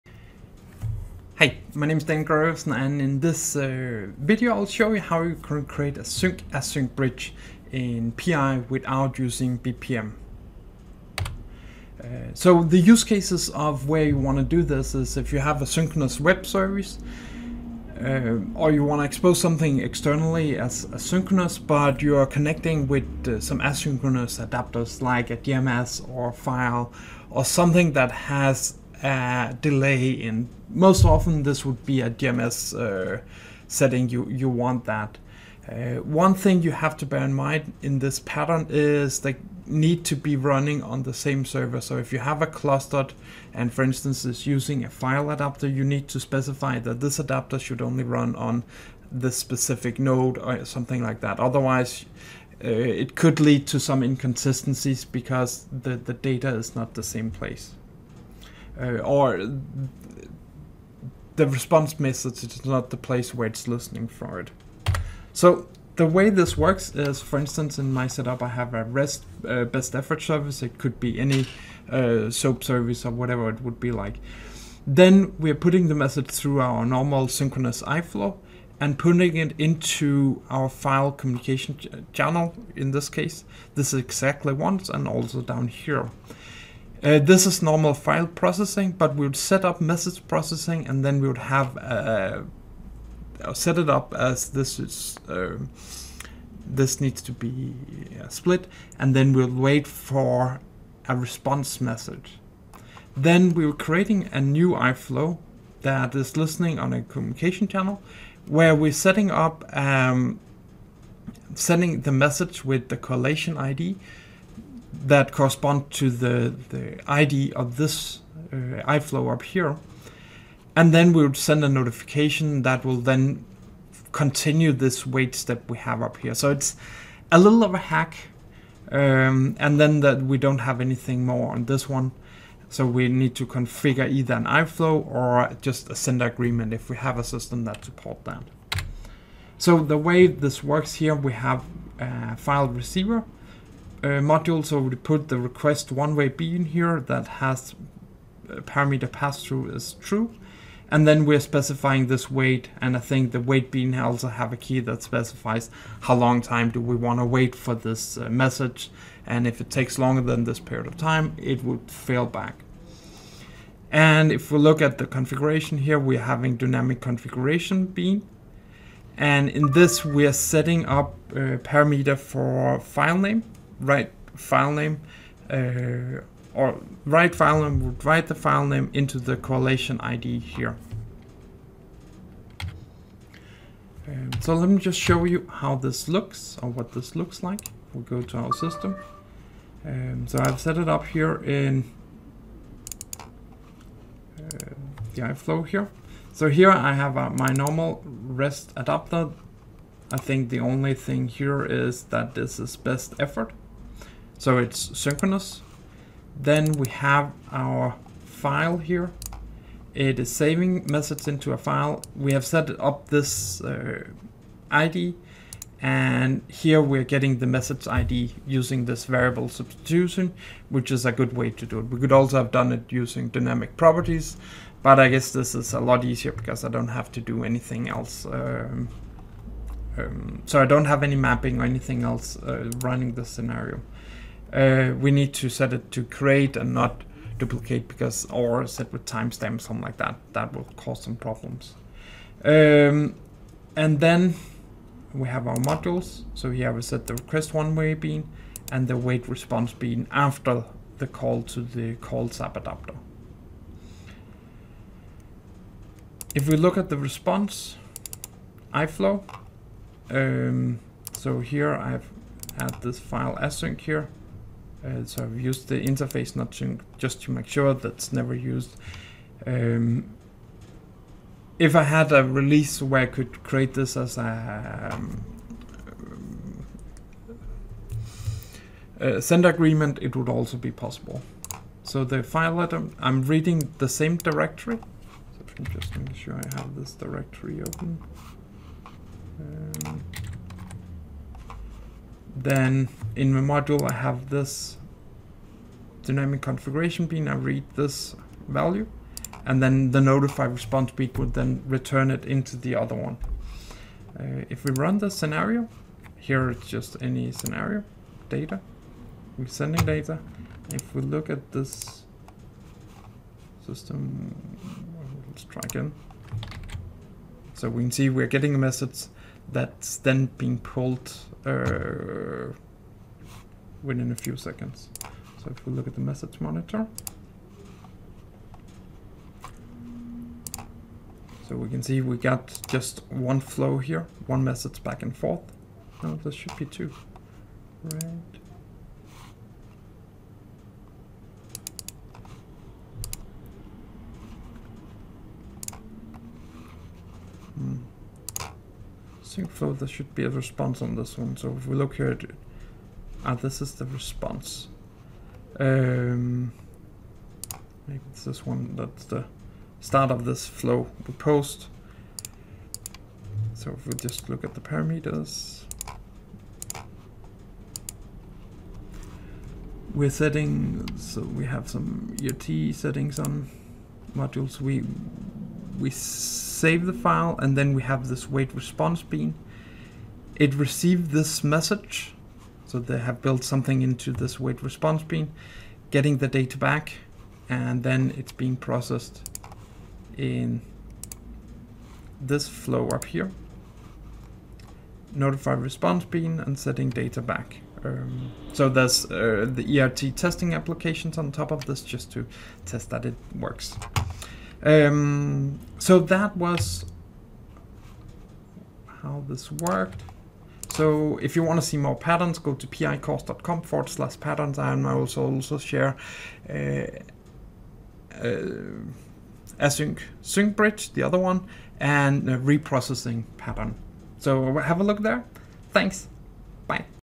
SAP PI/PO Sync Async bridge without BPM
This pattern describes how you can configure a sync async pattern in SAP PI/PO